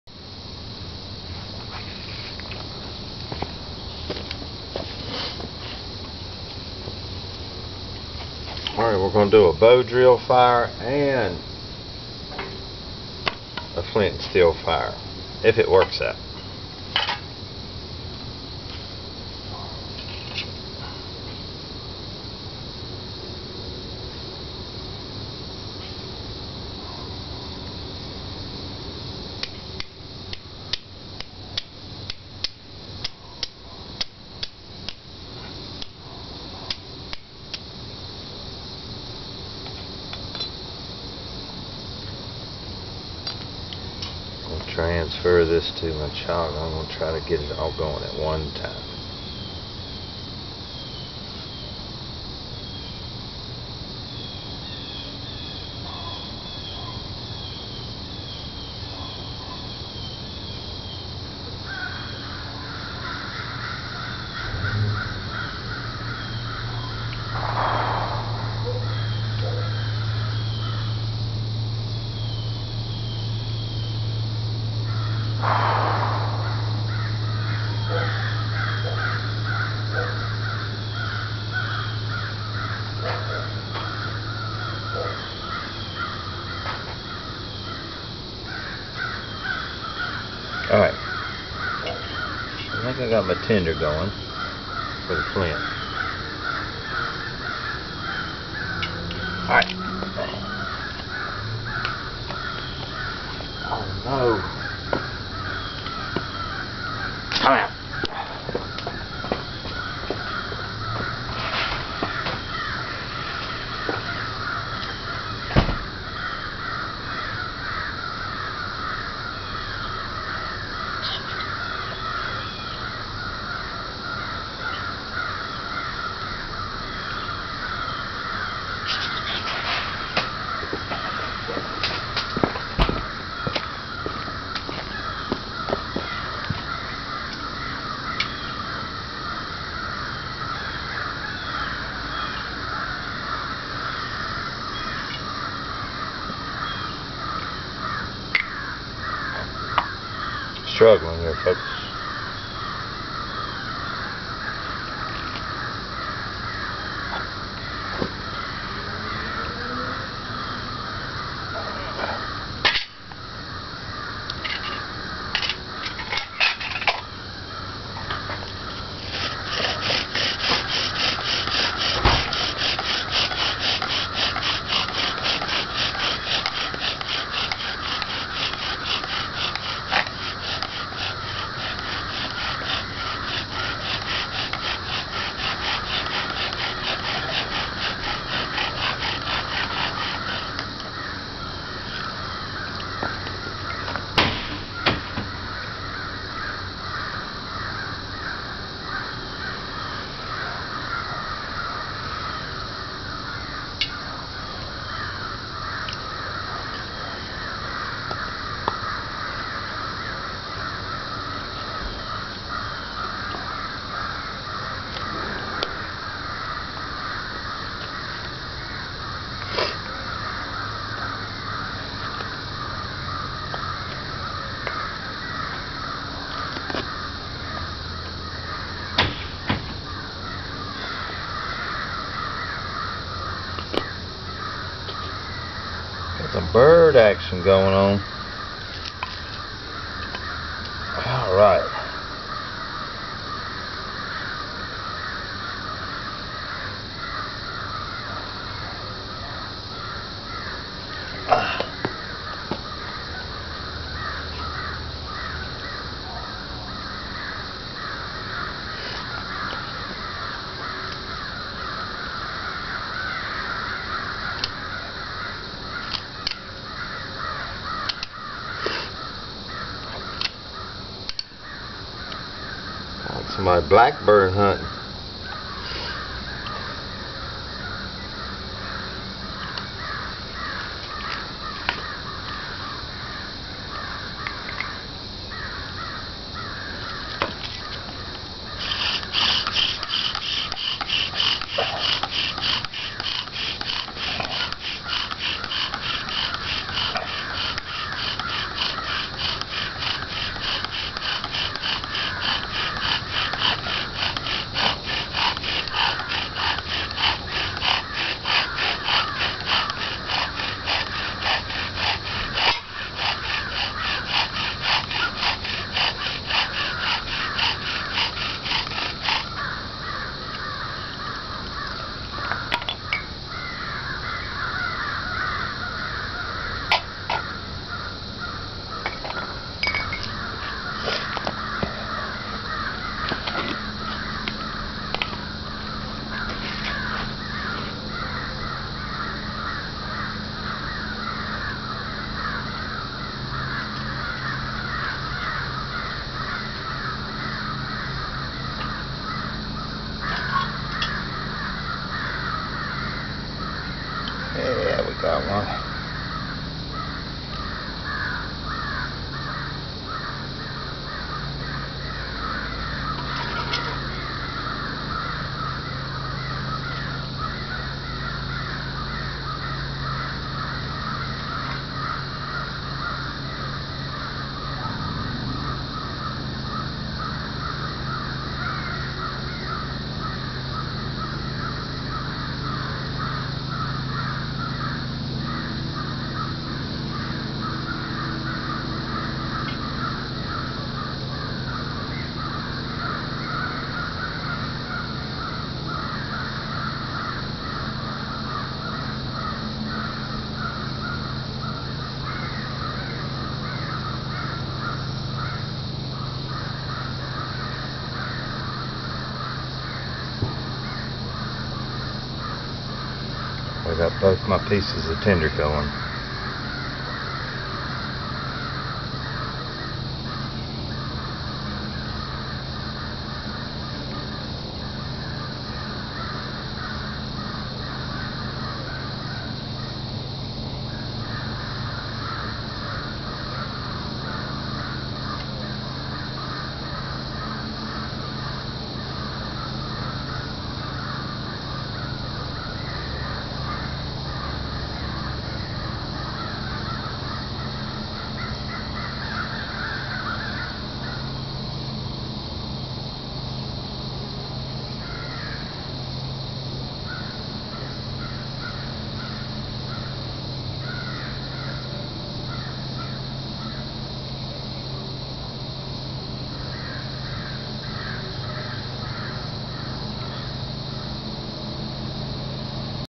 Survival/flint-steel & bowdrill exercise
As a challenge on Blades & Bushlore.com, I set out to start a flint/steel fire and bowdrill fire at the same time. I got ember from both but my camera went dead before I got both to flame. I'm charging my battery now and will do it again...I know. Pics or it didn't happen :).